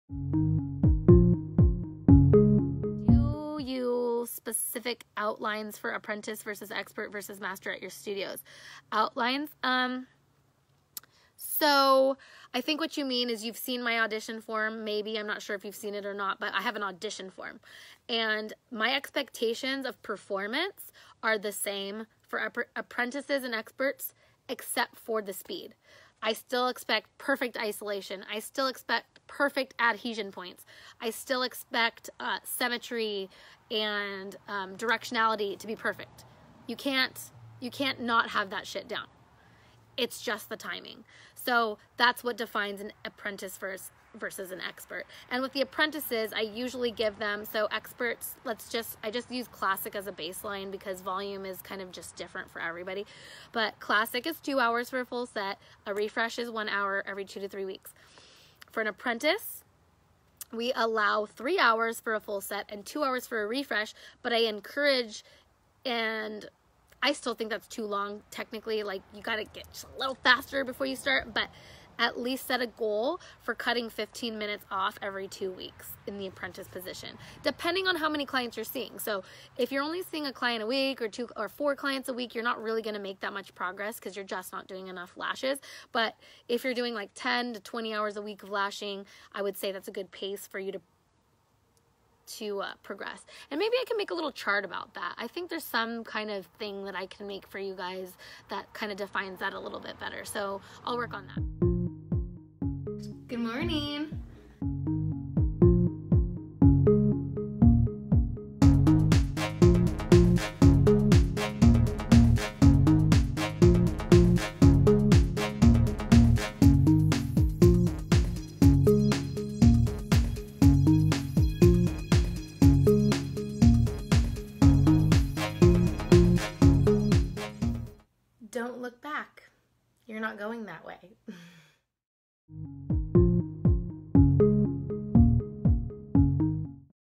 Apprentice vs. Expert Lash Artists
An Excerpt from LASHES+COFFEE LIVE #331,
A daily livestream available for Lash Business discussion and mentoring for Lash Artists.
We discuss all aspects of technique, tools, business and marketing!